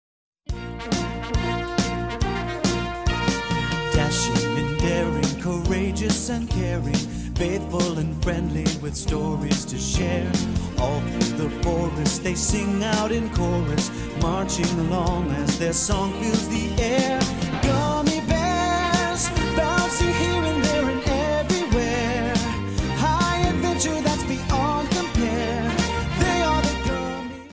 Rocky's Magic Journey
Rocky travels the forest in search of Gummi Berry juice! This dog is amazing!